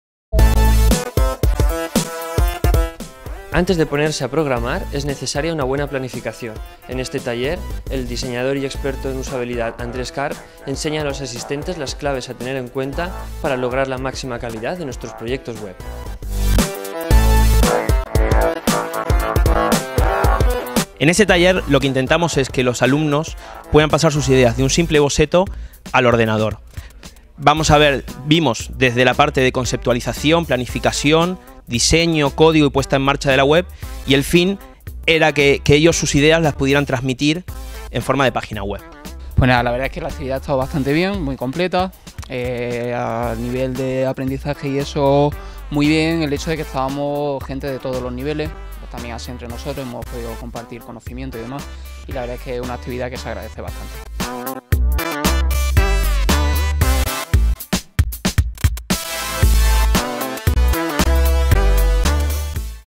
Taller: Del papel al ordenador
Andrés Karp explicó a los asistentes cómo pasar sus ideas del papel a los programas y las habilidades necesarias para publicar con éxito su propio proyecto en Internet.